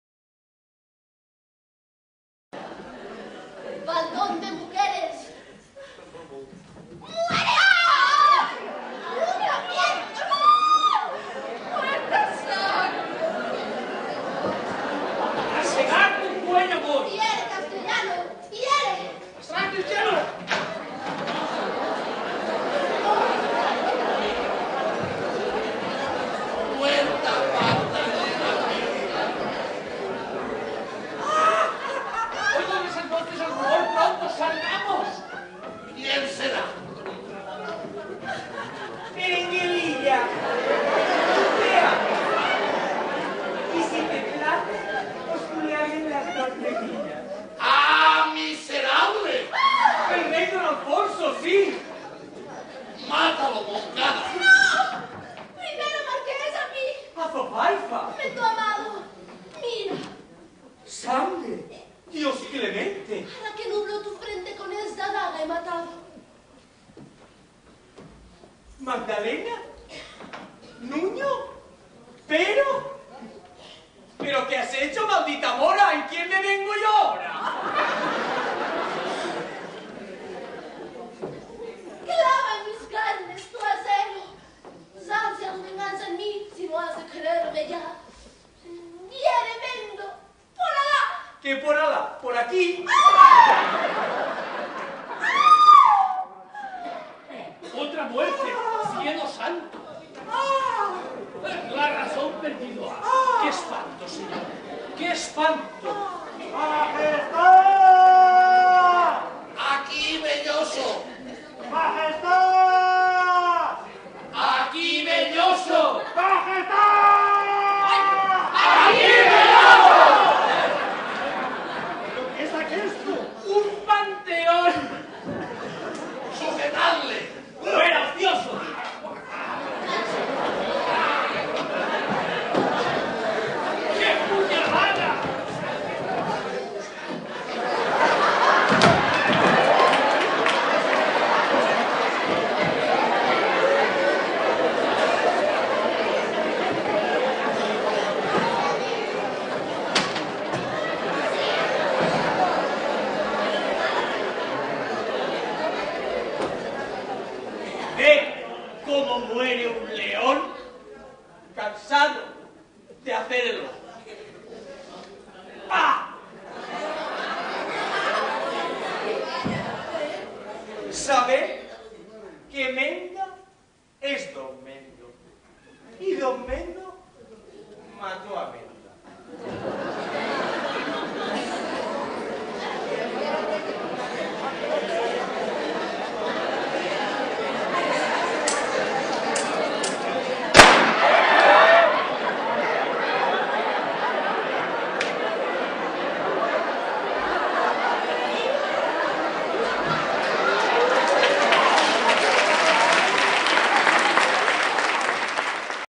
Ágora Grup de Teatre. Carcaixent
La Venganza de Don Mendo. Representada por este grupo desde el año 2000. Dirección de Vicente Giner. Filmación David y Carmina Carrillo. Edición para You Tube de Vicente Peralt